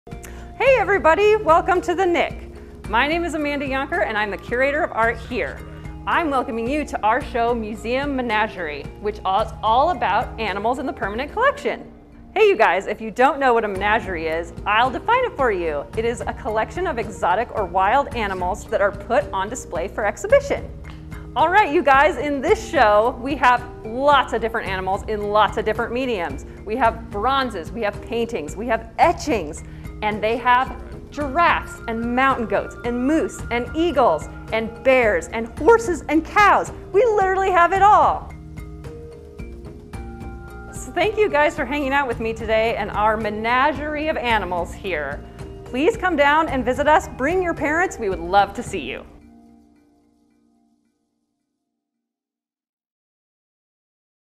2021 Museum Menagerie Education
Hear from our curator about the different elements in our Museum Menagerie exhibit.